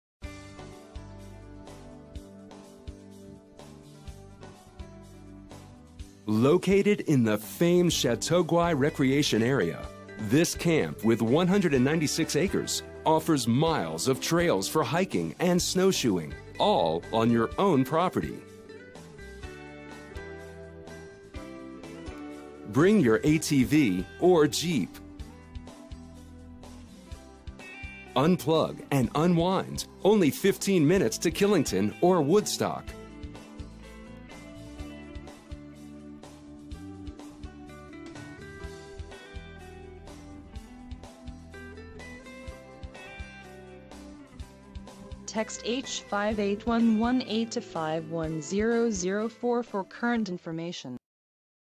4728 Chateauguay Road Bridgewater VT
Located in the famed Chateauguay recreation area,  this camp with 196 acres offers miles of trails for hiking and snowshoeing,  all on your own property  Bring your ATV or jeep  Unplug and unwind only 15 minutes to Killington or Woodstock

Video uploaded on Thu, 24 Oct 2013 13:17:56 -0500